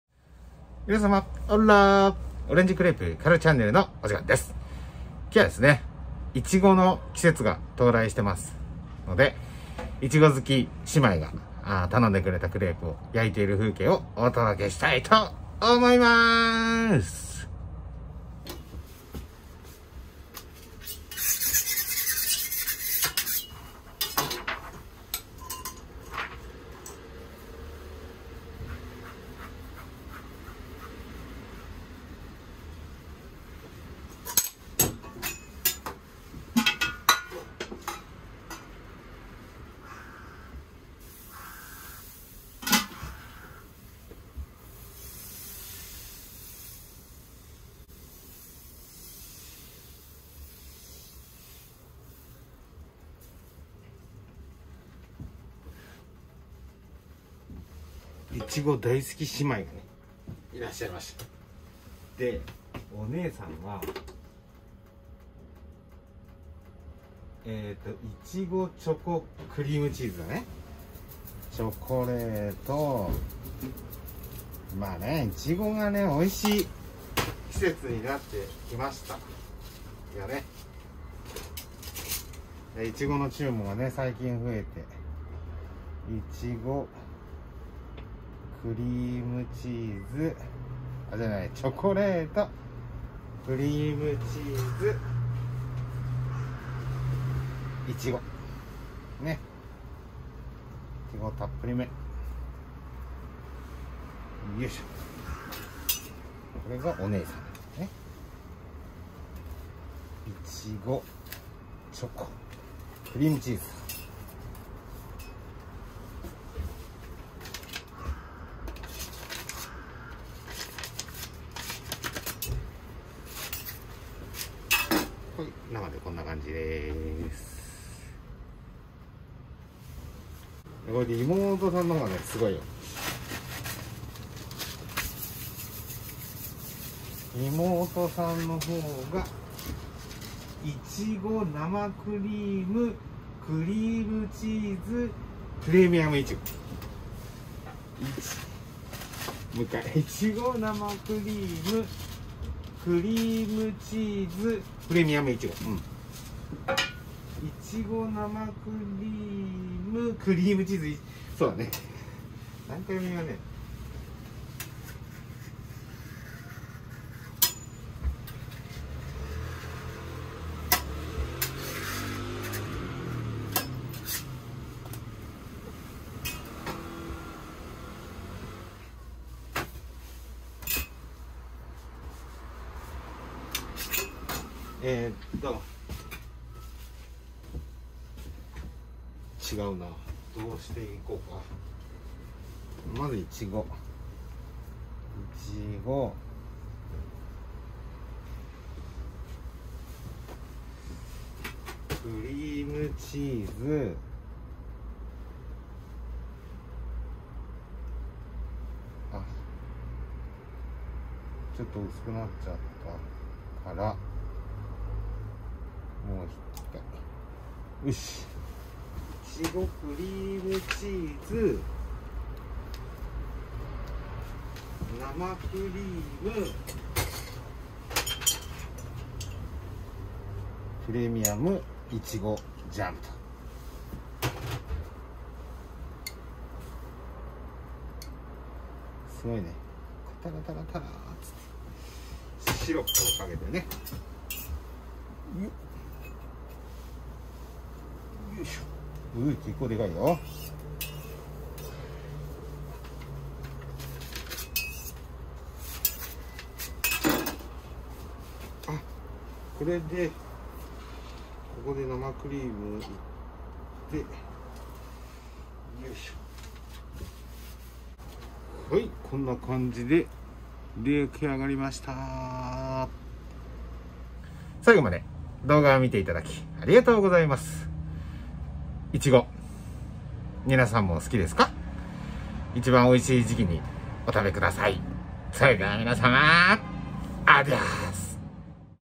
イチゴが美味しい季節です！色々な組み合わせで楽しもう！
イチゴが美味しい季節になってまいりました、皆様も、楽しく美味しく組み合わせ下さいませ！crepe